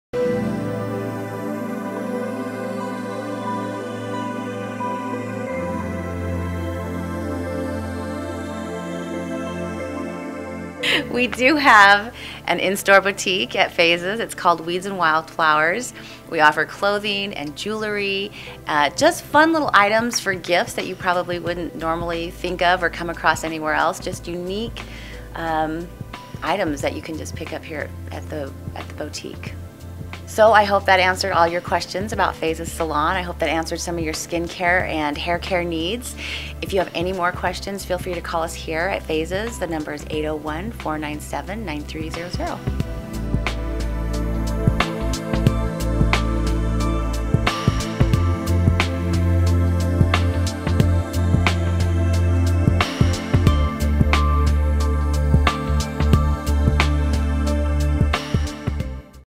Wildflowers and Weeds - In store boutique - Layton Massage
In this video, Karen, the Front End Coordinator of Phazes Salon, discusses about their in store boutique Wildflowers and Weeds

We do have an in store boutique at Phazes, it's called Weeds & Wildflowers.  We offer clothing and jewelry, just fun little items for gifts that you probably wouldn't normally think of or come across anywhere else, just some unique items that you can just pick up here at the boutique.

I hope that answered all your questions about Phazes Salon.  I hope that answered some of your skin care and hair care needs.